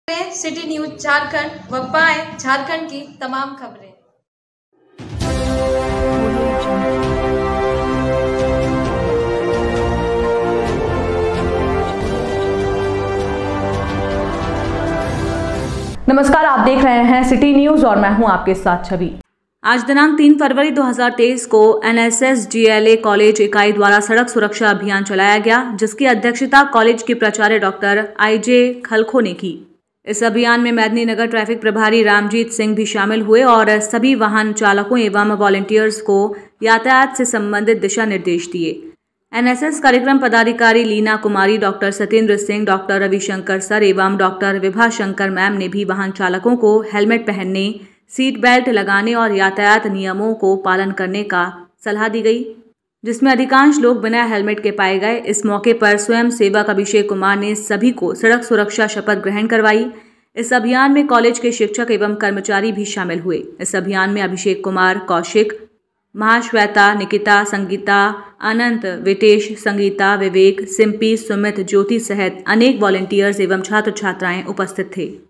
एन.एस.एस. जी.एल.ए. कॉलेज इकाई ने चलाया सड़क सुरक्षा अभियान
एन.एस.एस. जी.एल.ए. कॉलेज इकाई ने चलाया सड़क सुरक्षा अभियान#jharkhand

channel and I do not claim any right over any of the graphics, images, songs used in this video.